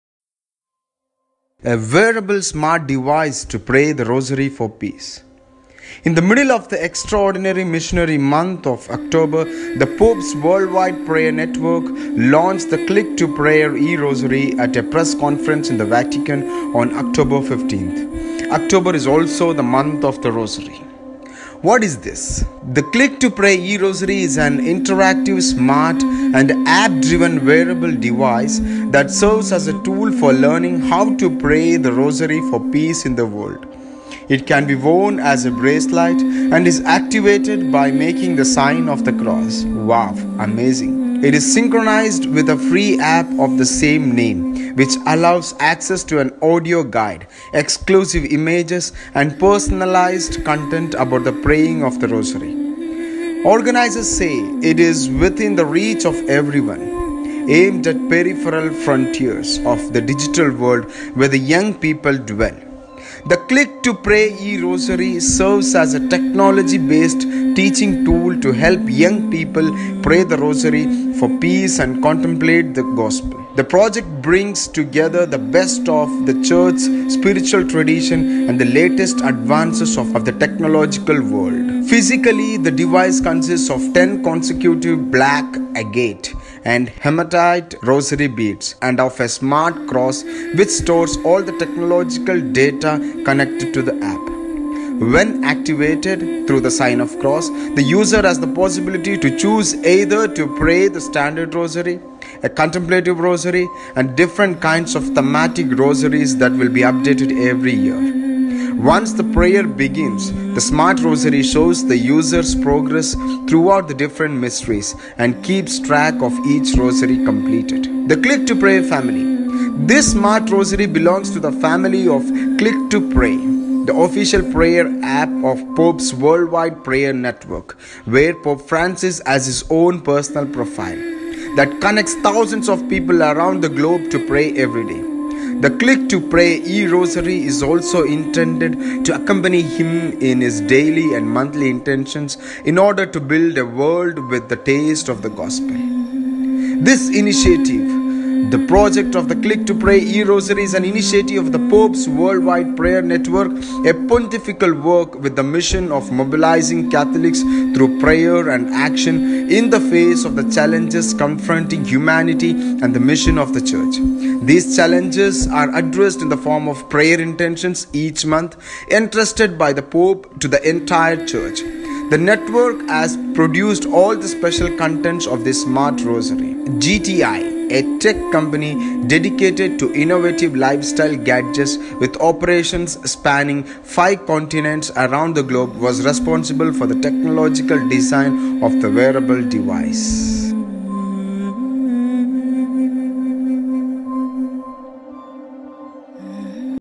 "Click to Pray eRosary” – wearable smart device to pray the rosary for peace
This smart rosary belongs to the family of “Click To Pray”, the official prayer app of the Pope’s Worldwide Prayer Network (where Pope Francis has his own personal profile) that connects thousands of people around the globe to pray every day. The Click To Pray eRosary is also intended to accompany him in his daily and monthly intentions in order to build a world with the taste of the Gospel.
 Pope’s Worldwide Prayer Network.